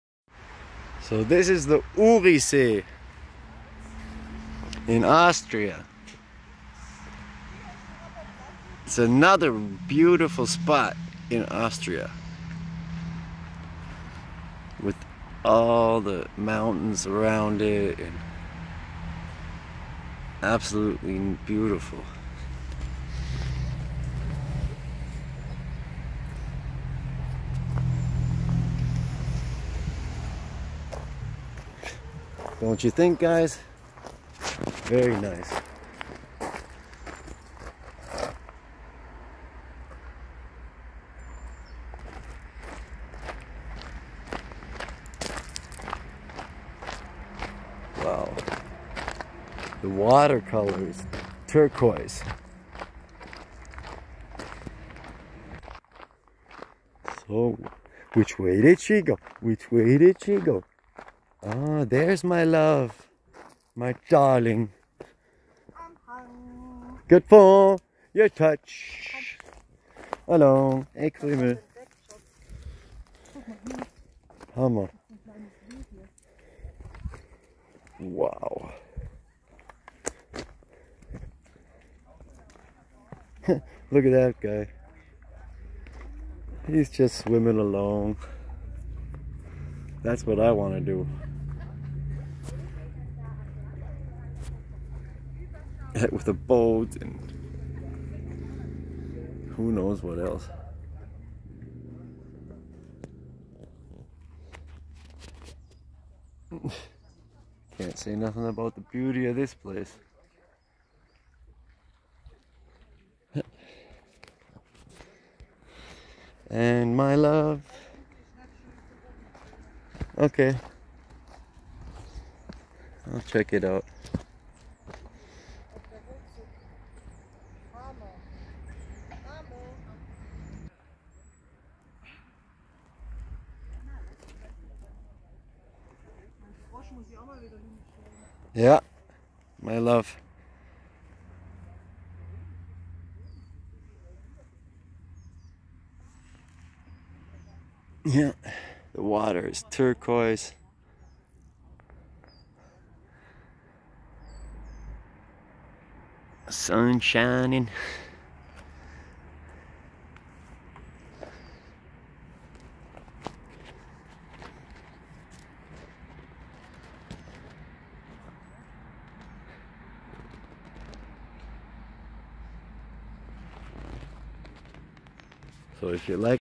URISEE LAKE AUSTRIA 🇦🇹 PART. 1
In this video I'm filming at the Urisee Lake in Austria. I was walking around the lake with my girl and our dog 🐕. The water was beautiful turquoise colored and the surrounding mountain range was like direct from a fairytale. absolutely amazing place. and we went on 2 C many other very nice places there in the surrounding area.